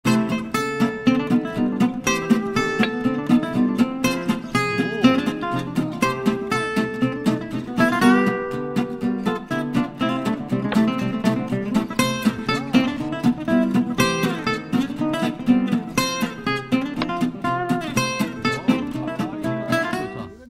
2015년 마지막 라운딩을 친구들과 함께
오늘 추울줄 알았는데 바람도 안불고 춥지않게 친구들과 마지막 라운딩을 김해 가야CC 에서 가졌습니다. 내기 없이 막걸리를 마시며 즐거운 한때를 보냈습니다. 슬로우 모션으로 찍으니 다들 프로같군요. 그냥 보면 고만 고만 한데.. ㅎㅎ